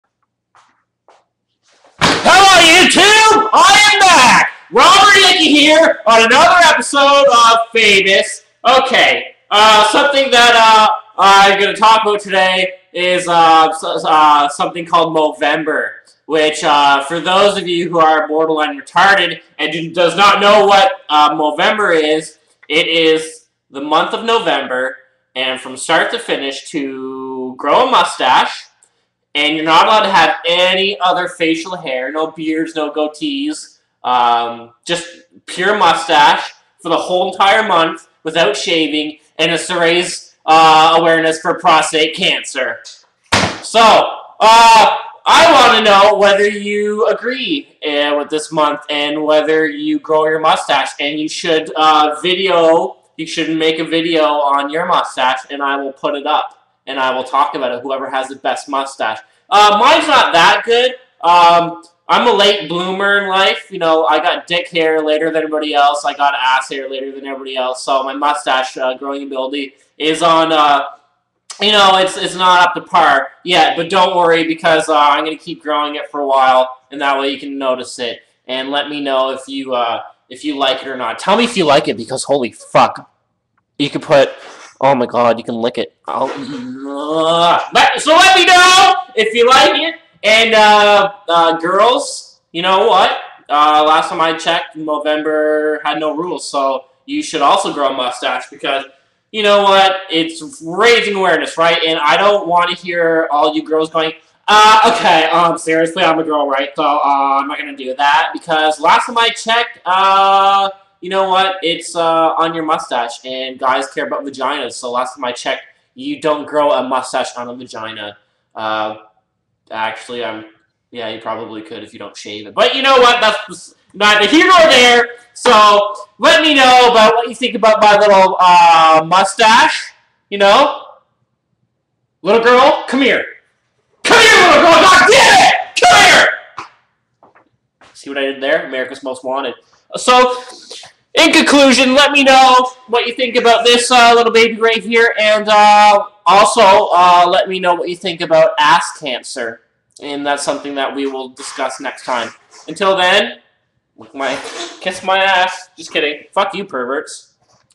MOVEMBER!
Todays video is about the Movermber month and what its all about bitches.